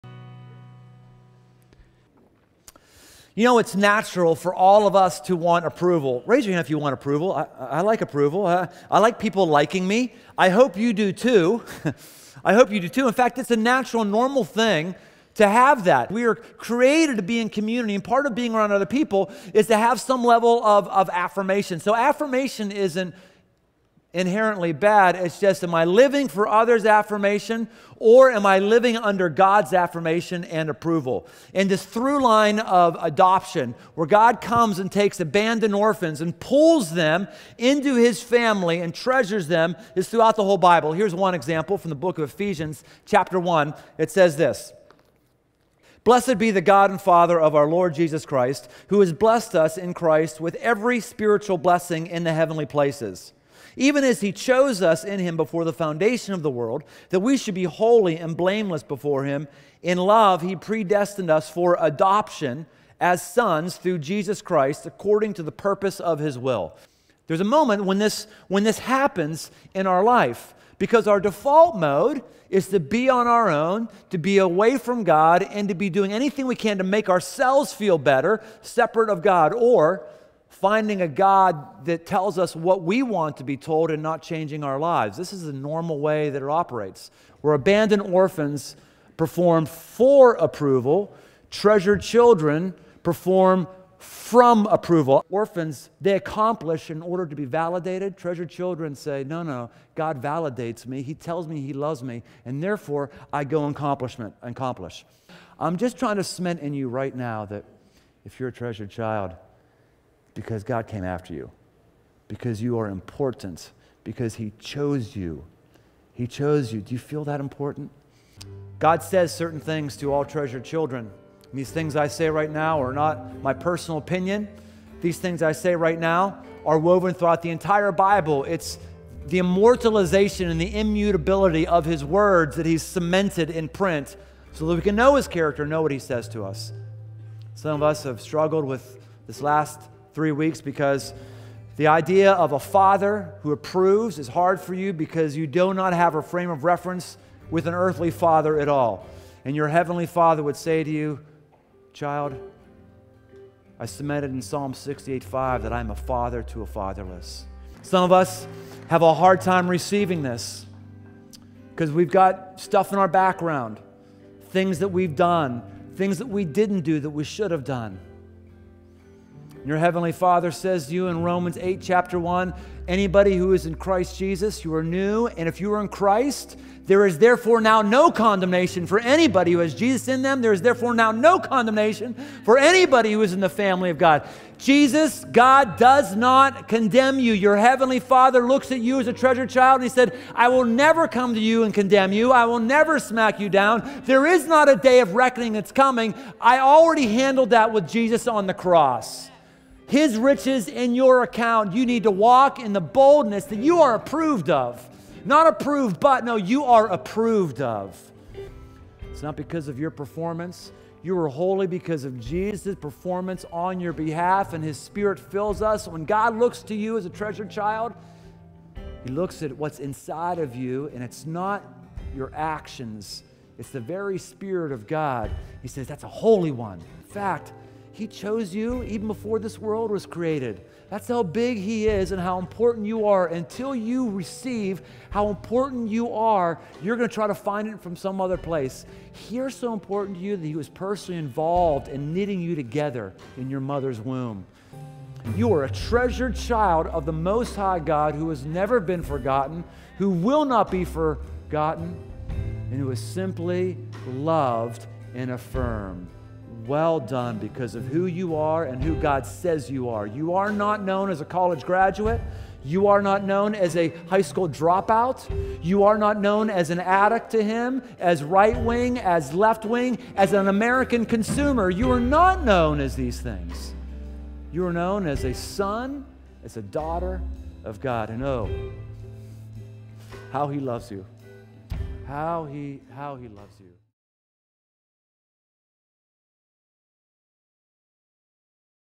A Place at the Table: Week 3 Summary (Finding Approval)
No matter our history or family of origin, we can all feel abandoned and alone. In the midst of life's challenges, it's easy to default to operating as emotional and spiritual orphans. In this third weekend of our series, Brian Tome discusses our need for approval, and how treasured children of God perform FROM approval, not for it.